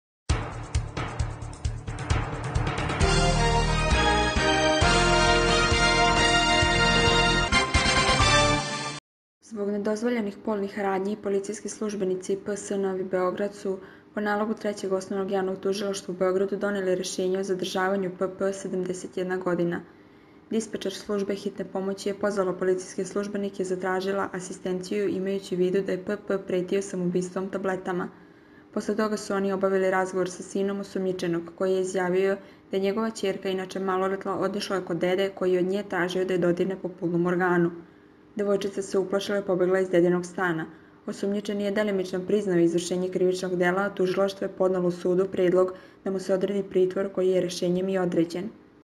DEDA 71 TRAŽIO OD UNUKE DA GA DIRA PO POLNOM ORGANU U Tužilaštvu delimično priznao delo
DEDA (71) TRAŽIO OD UNUKE DA GA DIRA PO POLNOM ORGANU: U Tužilaštvu delimično priznao delo, određen pritvor